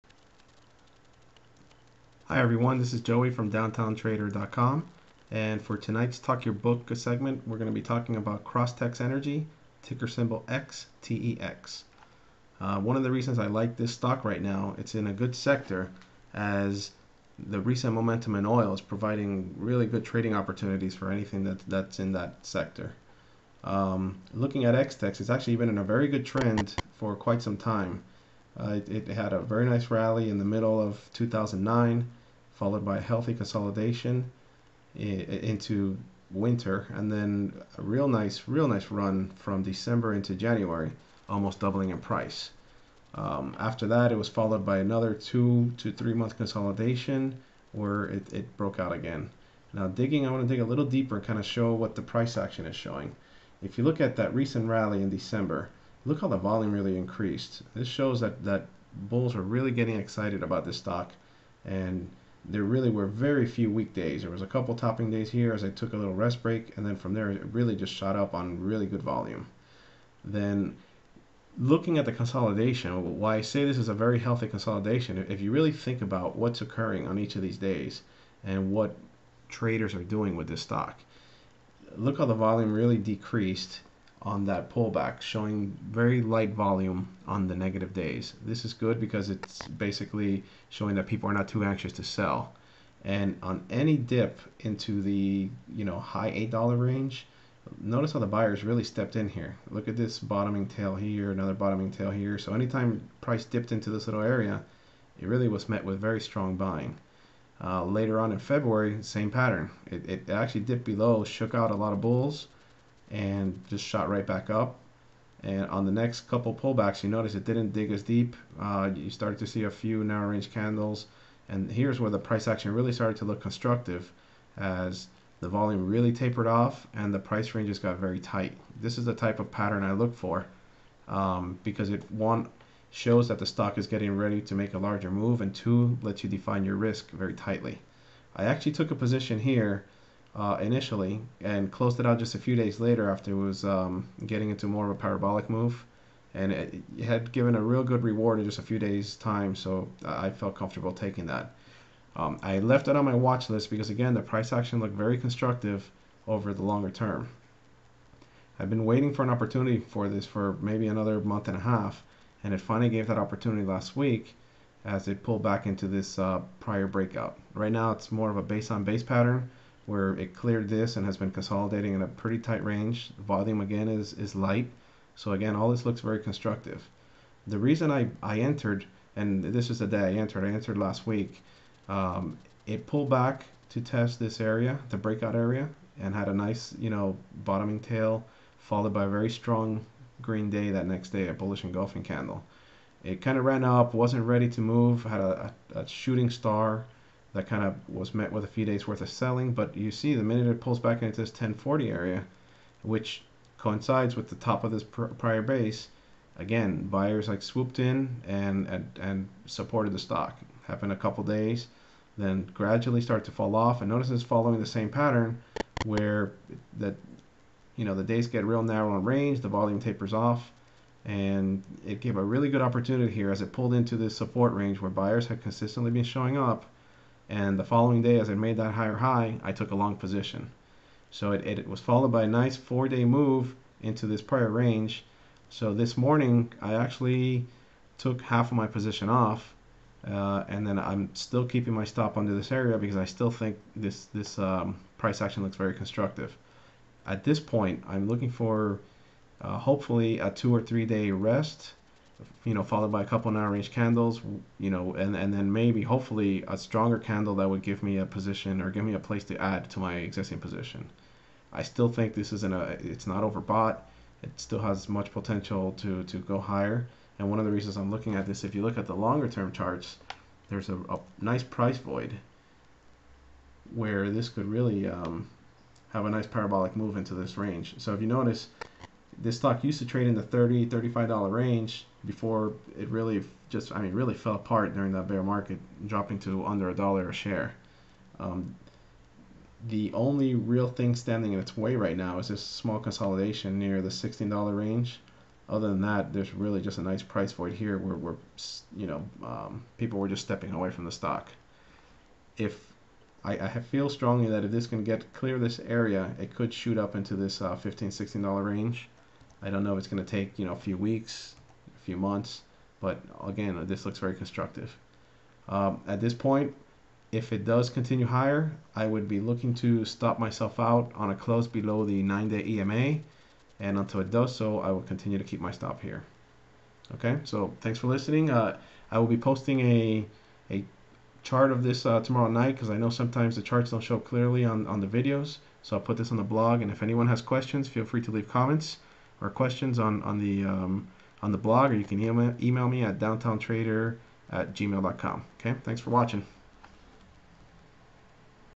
Talk Your Book - XTEX
Talk your Book segment discussing a trade and possible setup in Crosstex Energy, L.P.
(Public, NASDAQ:XTEX) .